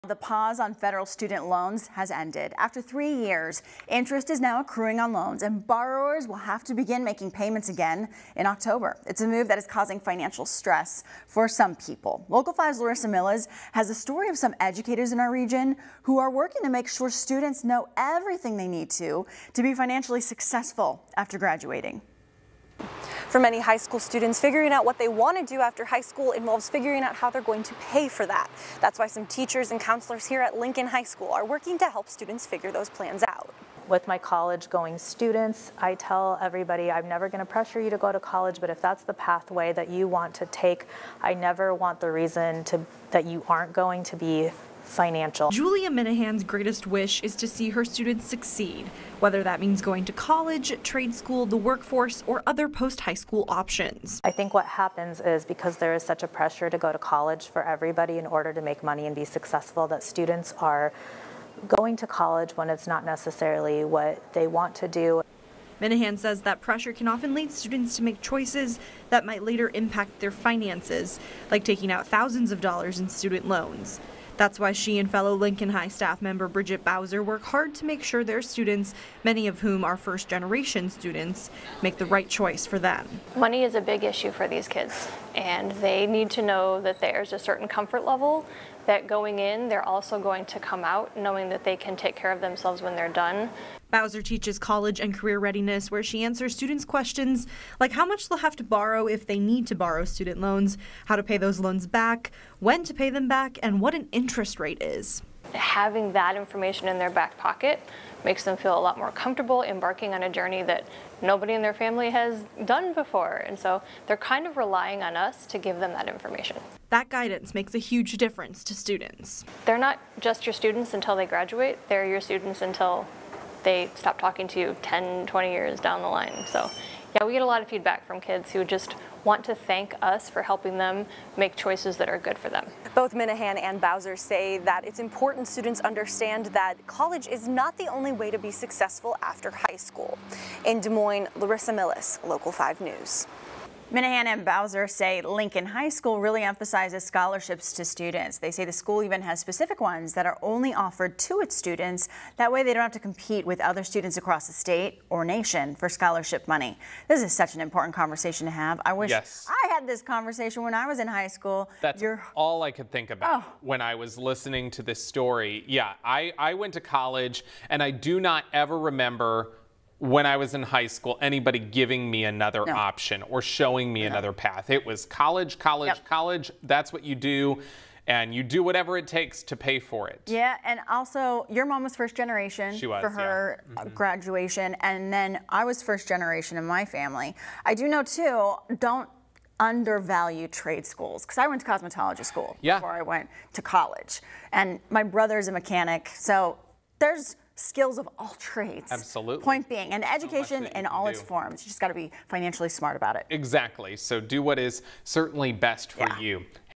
'Money is a big issue for these kids': Learning about financial literacy while in high school
The pause on federal student loans has ended.

After three years, interest is now accruing on loans and borrowers will have to begin making payments again in October.

It's a move that is causing financial stress for some people.